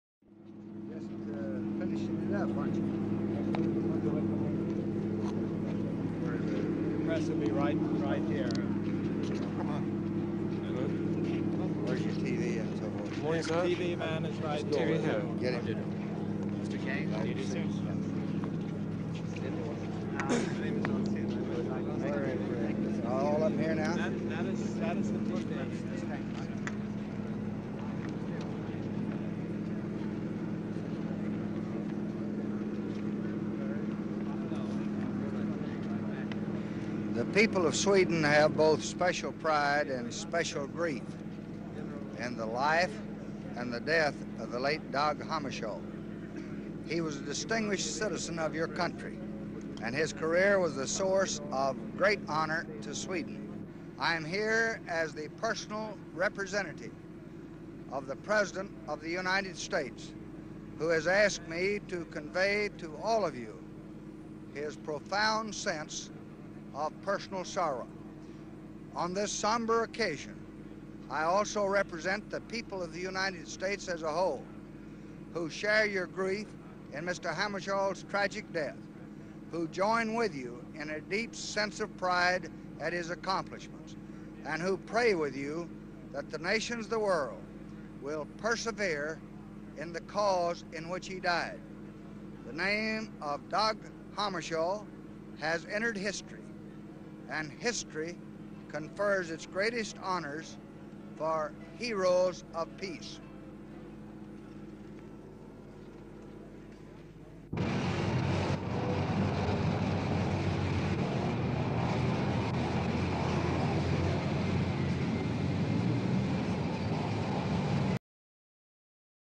September 29, 1961 - Vice President Lyndon Johnson arriving Stockholm for Dag Hammarskjöld's funeral
Vice President Lyndon B. Johnson and US Ambassador of the United Nations Adlai Stevenson arriving Arland Airport, Stockholm for the funeral of Dag Hammarskjöld. They are met by Swedish Prime Minister Tage Erlander.
Letters to the Vice President Relating to His Trip to Sweden To Attend the Funeral of Dag Hammarskjold.
''Dear Lyndon:

I am very grateful to you for undertaking to serve as my representative at the funeral of Dag Hammarskjold. I would have liked to make this journey myself, and I am most grateful that you will act as the senior representative for the United States in my place. I am sure you will convey to the Hammarskjold family and to the appropriate officials of the Swedish government my own profound sympathy and respect of the American government and the American people.

Sincerely,

Dear Lyndon:

I know that your trip to Sweden will perhaps be brief and that you might find it necessary to come back immediately after the Hammarskjold funeral because of previous engagements, but if circumstances make it possible I shall be glad if you can find time for a brief stop in Paris on your way back. There is much going on in our discussions with our major European allies and I shall be glad to have the advantage of your own account of discussions which you might have in Paris with our senior people there--men like Gavin, Finletter and General Norstad, if he has not already left to come back here for direct discussions in Washington.

Sincerely,